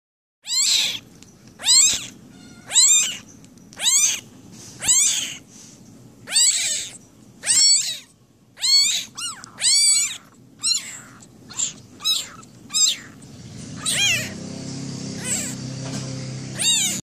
Bottle fed kittens, siamese
These little ones joined us as orphan and love to cuddle!

They both grew up and have found their forever families!